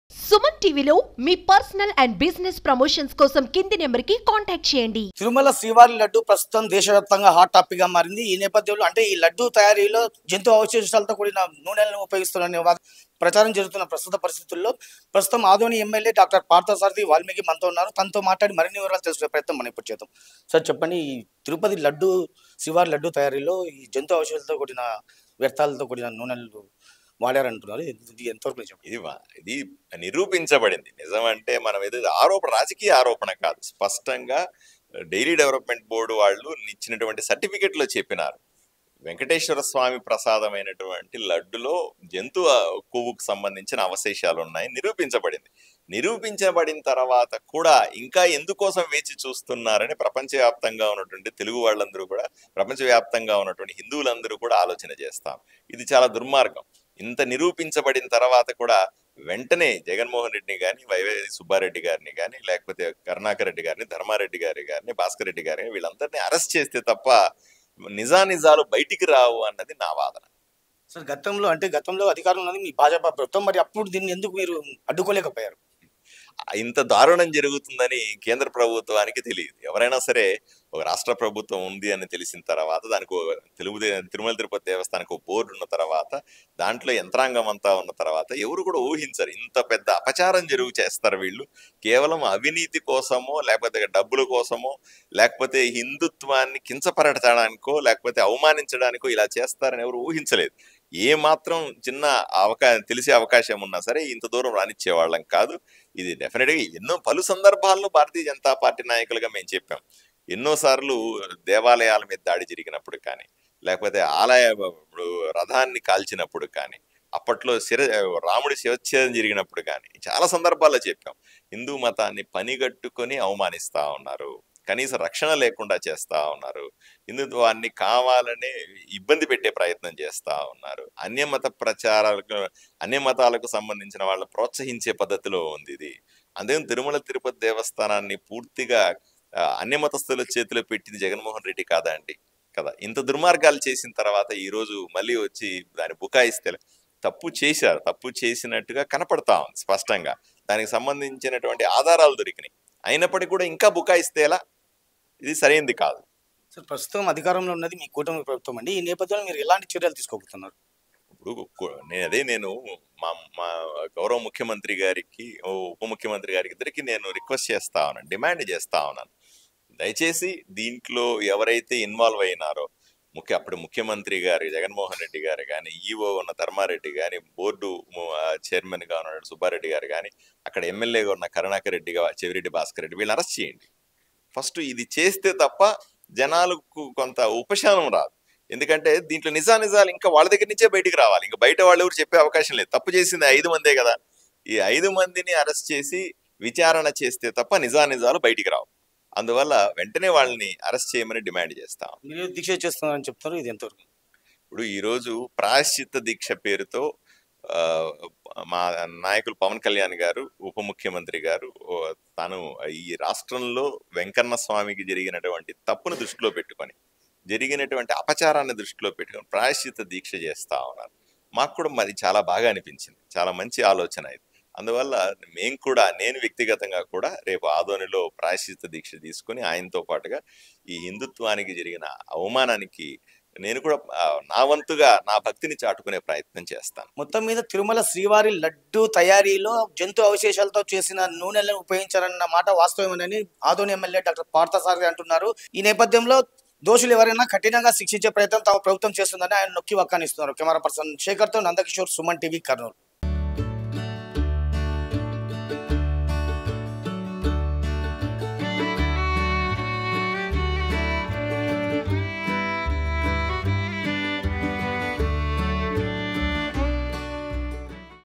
kurnool Adoni MLA Pardasaradhi About Animal Fat Oil In Tirupati Laddu | YS Jagan
Watch► kurnool Adoni MLA Pardasaradhi About Animal Fat Oil In Tirupati Laddu | YS Jagan  @SumanTVKurnool